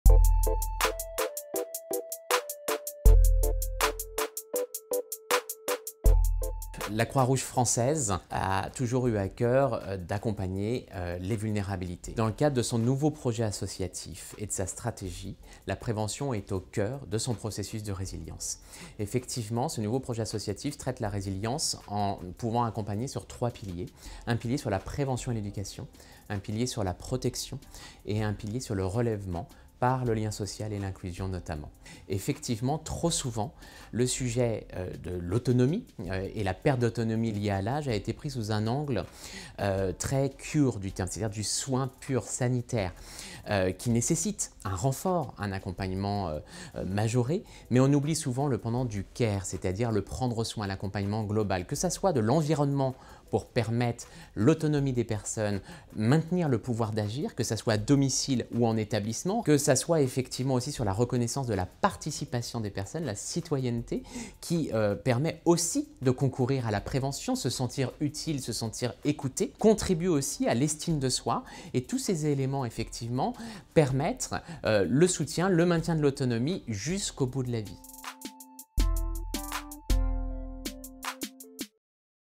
Johan GIRARD (Croix-Rouge française) - Prévention de la perte d’autonomie - CESE
Questions à M. Johan GIRARD, Directeur délégué à la coordination des politiques publiques des métiers de la Croix-Rouge française, auditionné par la Commission Affaires sociales et santé du CESE dans le cadre de la saisine : "La prévention de la perte d’autonomie liée au vieillissement".